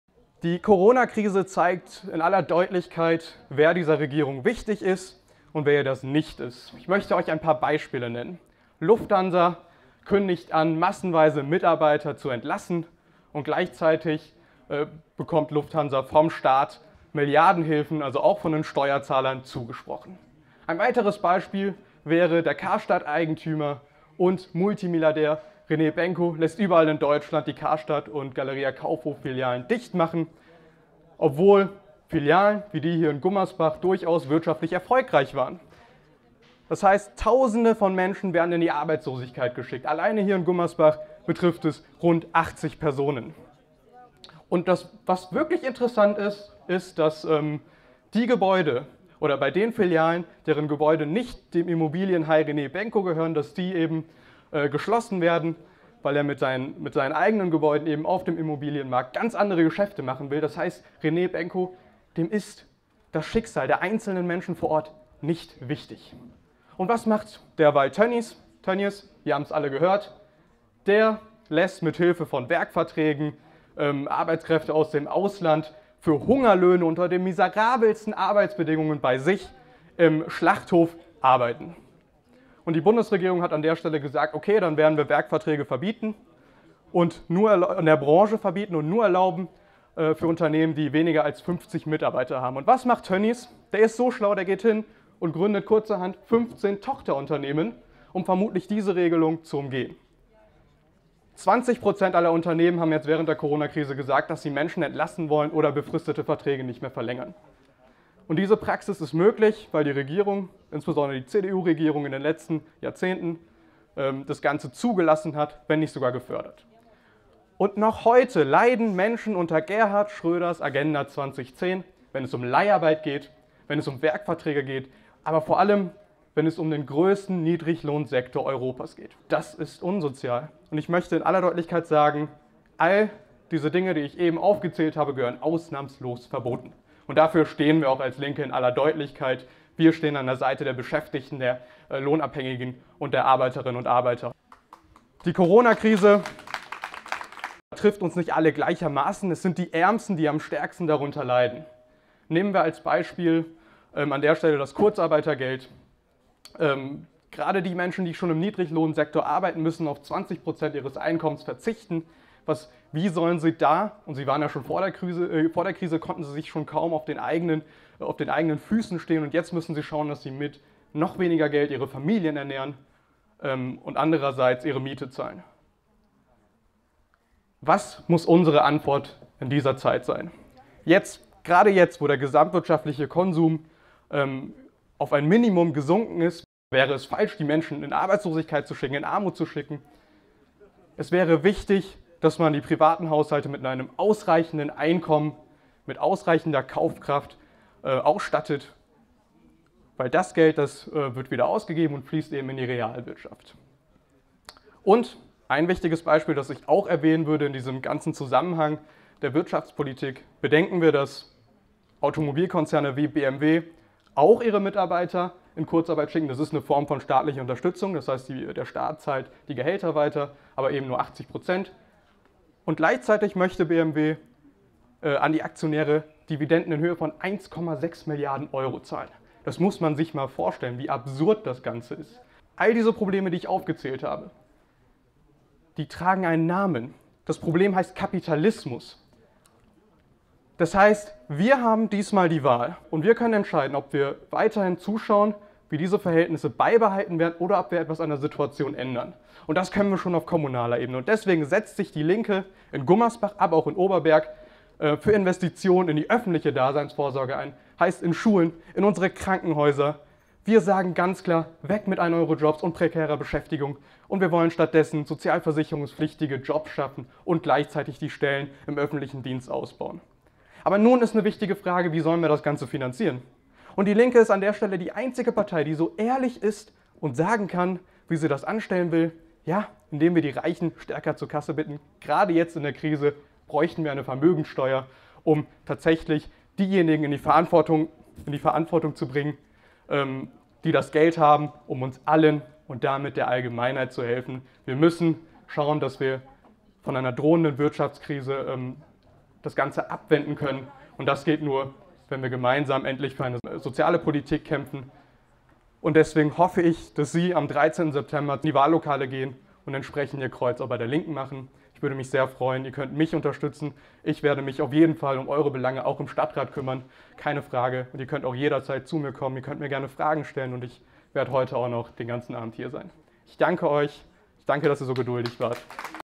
Corona-Krise zeigt: Der Kapitalismus ist das Problem | DIE LINKE | Kommunalwahl Gummersbach
Am 13. September finden in NRW Kommunalwahlen statt. Ich trete für den Rat der Stadt Gummersbach an.

Am 13. September haben wir die Wahl. Wir können entscheiden, ob wir eine unsoziale und kapitalistische Politik, die Menschen ausbeutet, behalten oder etwas an der Situation ändern. Und das ist schon auf kommunaler Ebene möglich.
Wir als Linke kämpfen in Gummersbach und Oberberg für Investitionen in die öffentliche Daseinsvorsorge, in unsere Schulen, unsere Krankenhäuser.
Wir wollen weg von 1-Euro Jobs und prekärer Beschäftigung und neue sozialversicherungspflichtige Jobs schaffen, wir wollen die Stellen im öffentlichen Dienst ausbauen.
Aber all das kostet nun mal Geld. Und DIE LINKE ist die einzige Partei, die sich traut auszusprechen, wie wir das alles finanzieren wollen.
In dem wir die Reichen zur Kasse bitten, gerade jetzt in der Krise, brauchen wir eine Vermögenssteuer, die vor allem die zur Verantwortung zieht, die das Geld haben, um uns allen und damit der Allgemeinheit zu helfen. Uns allen gehört die Stadt.